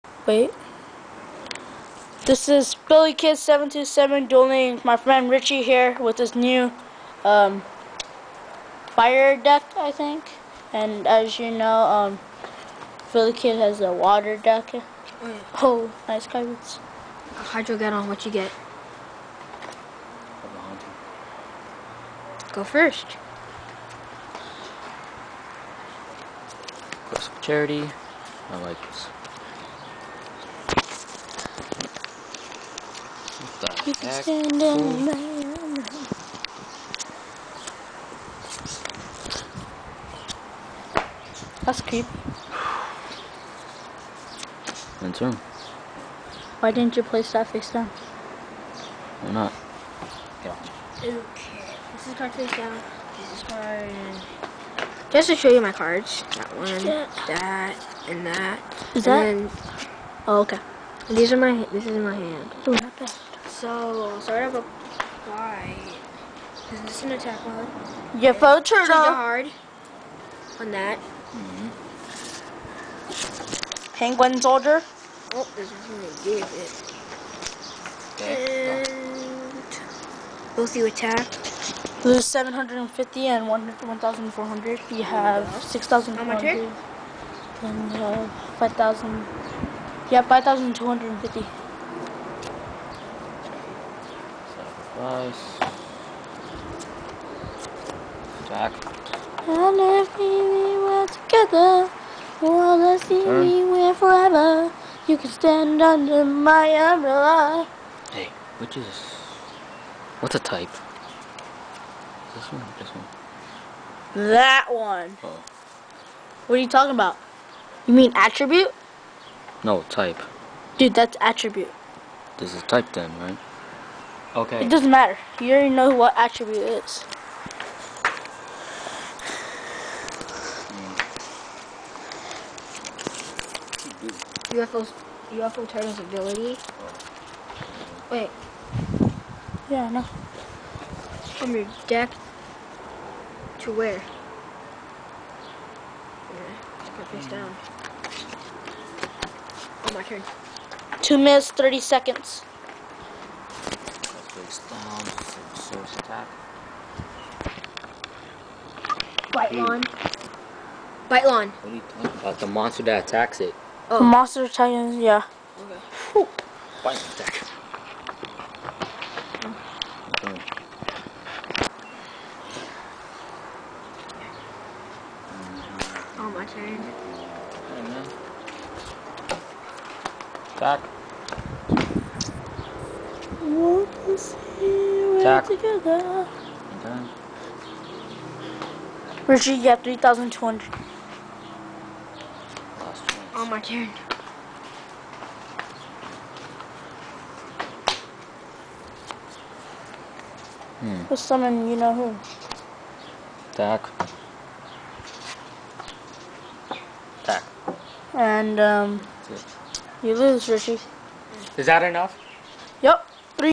Filikid vs Ritchie water vs fire deck
I finally manage to get a video of my friend Ritchie dueling he had made a new deck you could guess who won.